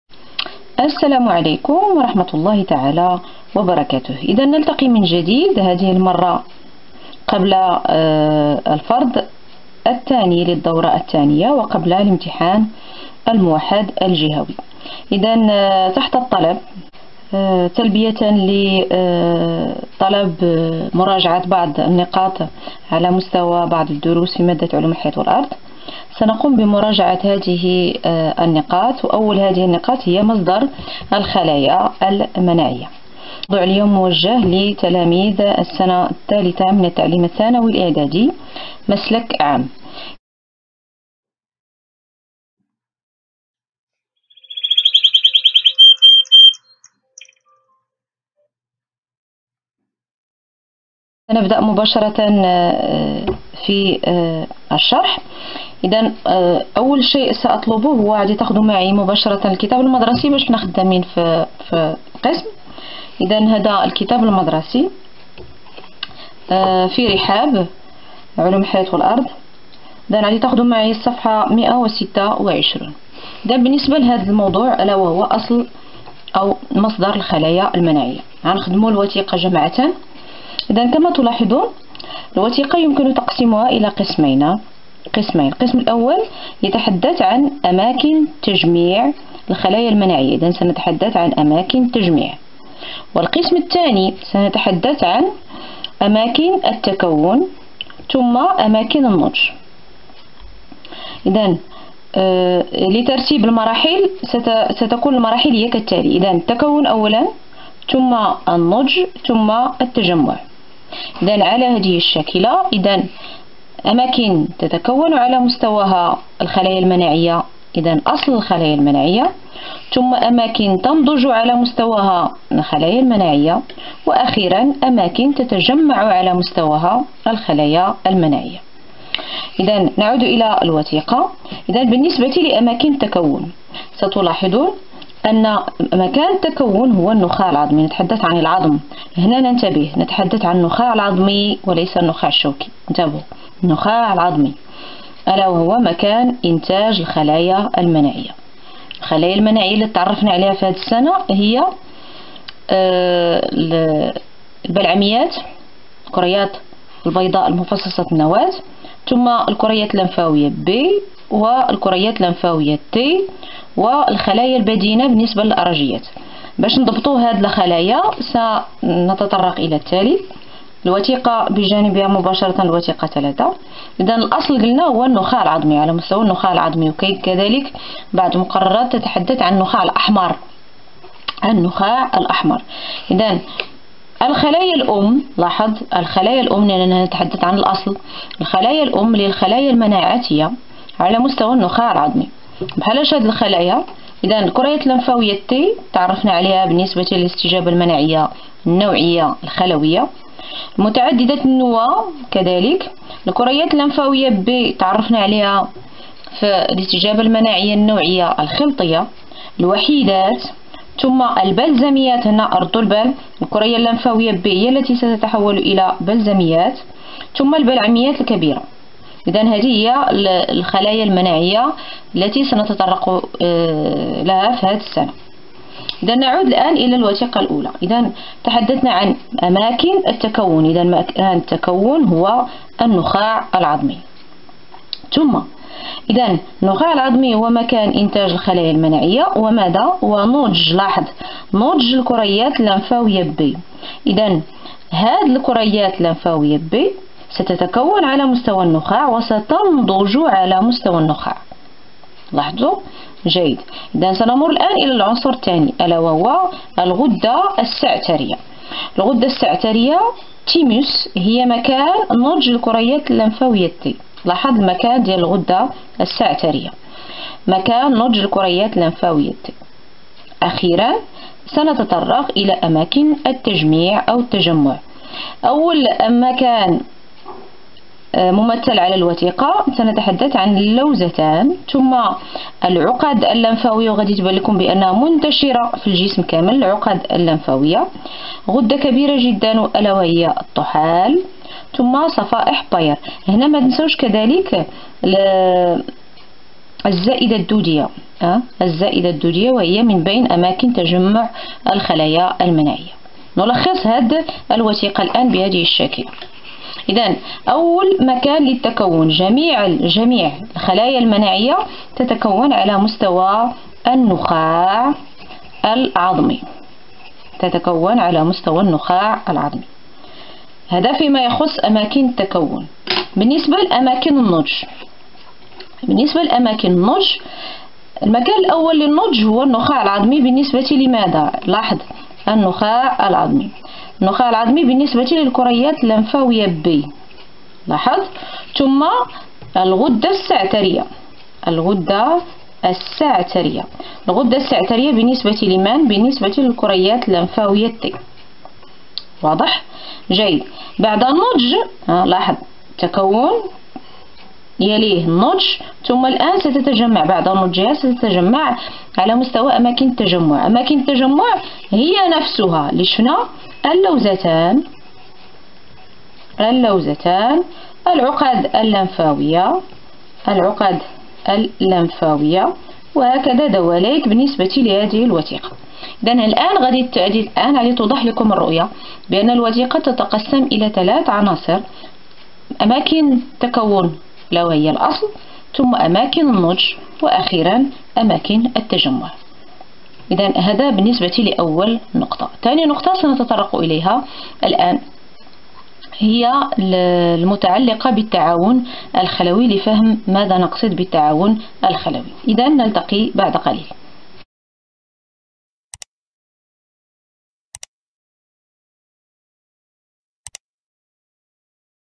أصل الخلايا المناعية - السنة الثالثة من التعليم الثانوي الإعدادي مسلك عام
شرح مبسط لدرس أصل الخلايا المناعية - إستعدادا للفرض الثاني للدورة الثانية والإمتحان الموحد الجهوي